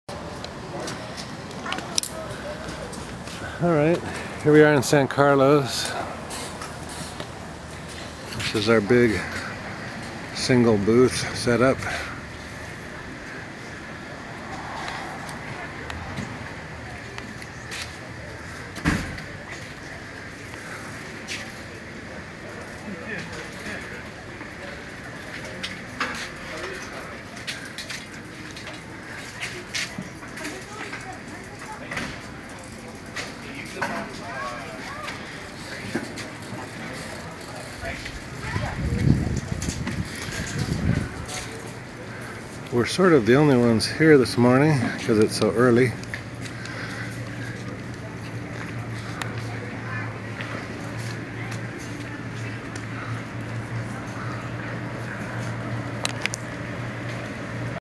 J&M Photography at the San Carlos Art and Wine Festival
This is a quick look at the booth Sunday morning on the last day of the fair, and our last art fair of the 2012 season.